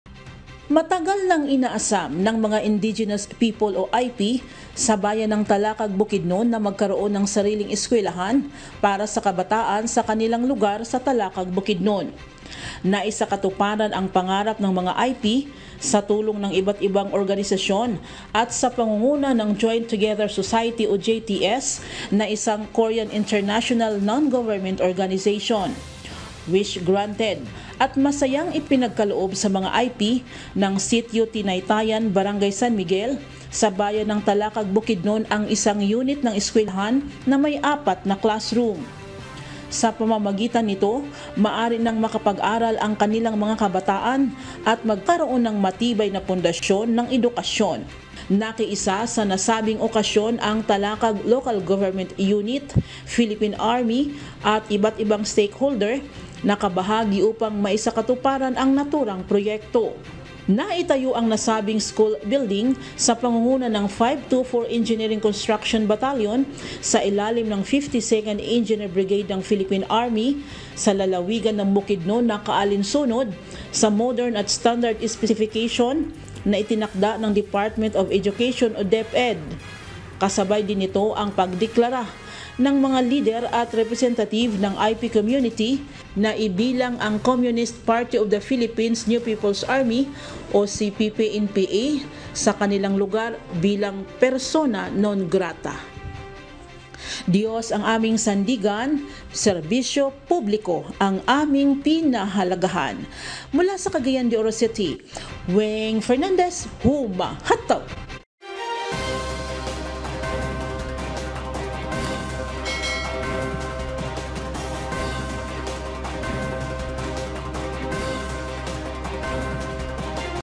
Pangarap na paaralan ng mga indigenous people sa Bukidnon, naipagkaloob na
Naipagkaloob na sa indigenous people community sa bayan ng Talakag, Bukidnon ang matagal na nilang pangarap na magkaroon ng paaralan sa kanilang lugar.